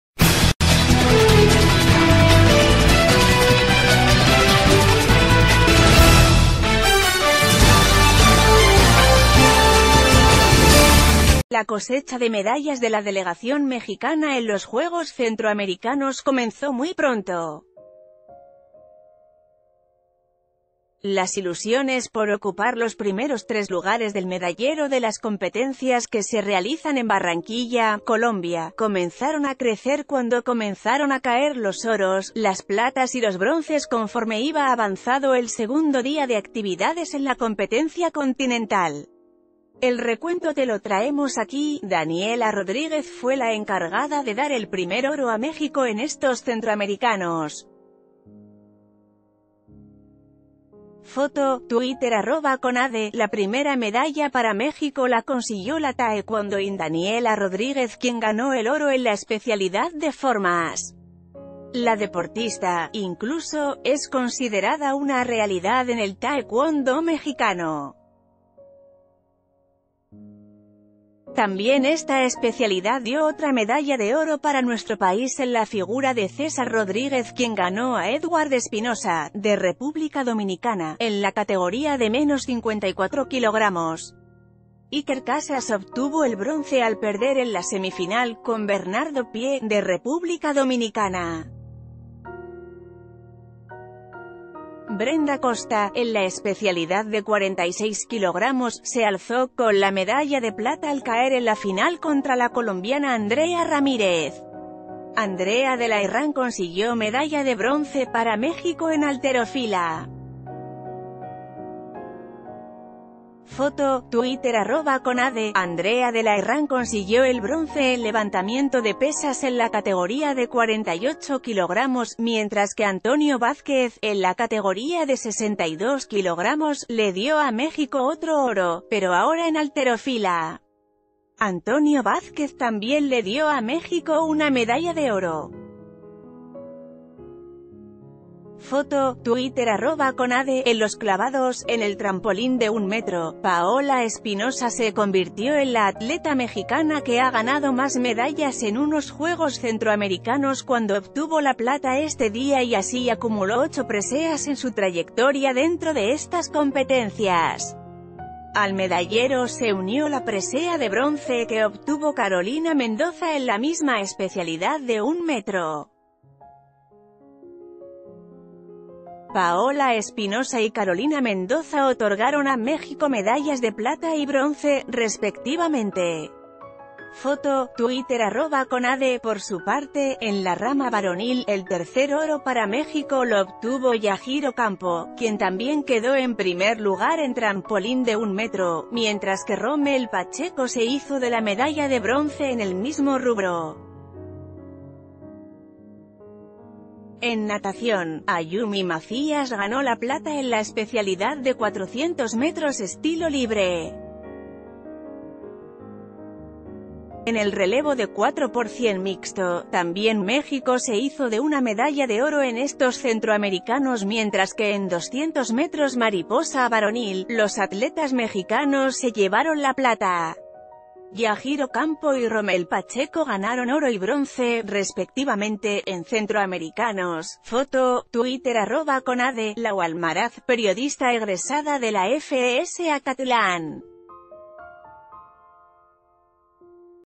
¡Llegaron las medallas para México en Juegos Centroamericanos!
La cosecha de medallas de la delegación mexicana en los Juegos Centroamericanos comenzó muy pronto. Las ilusiones por ocupar los primeros tres lugares del medallero de las competencias que se realizan en Barranquilla, Colombia, comenzaron a crecer cuando comenzaron a caer los oros, las platas y los ...